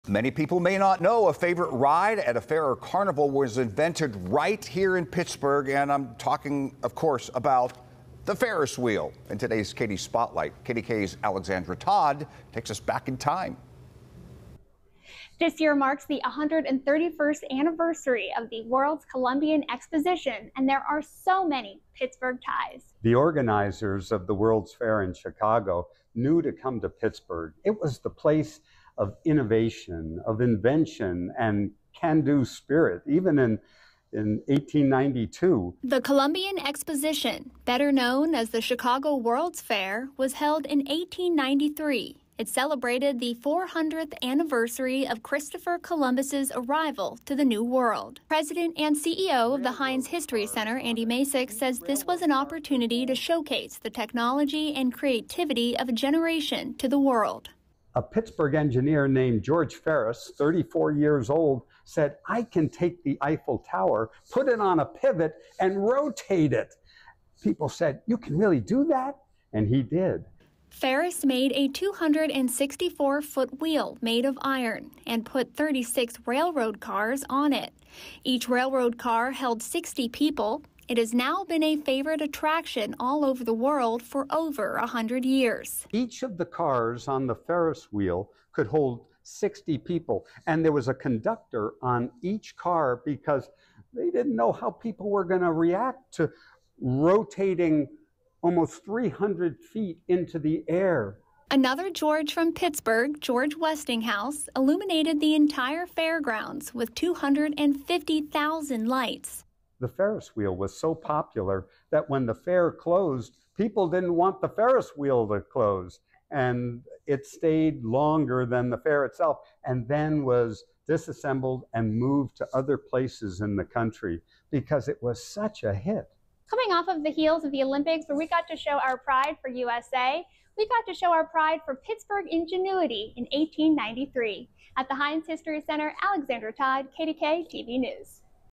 KD Sunday Spotlight: The Ferris Wheel & Pittsburgh
More than 100 years ago, the Ferris Wheel debuted on the world's stage but did you know it was invented here in Pittsburgh? Alexandra Todd explains in the KD Sunday Spotlight!